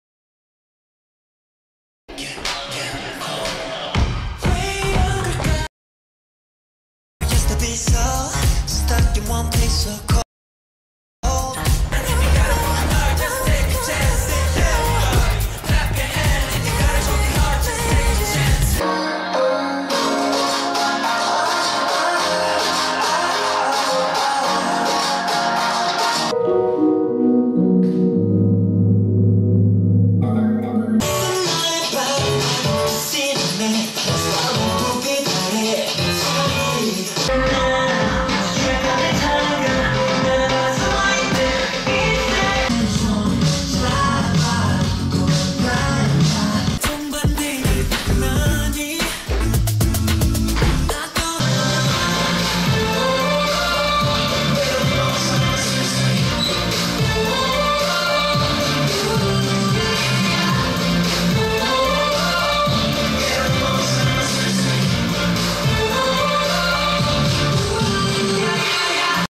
Tell me they are TXT without telling me "They are TXT"
Just my guess based on their choreography, we can see TXT's shape themselves in some moves.
Hope you have fun in watching !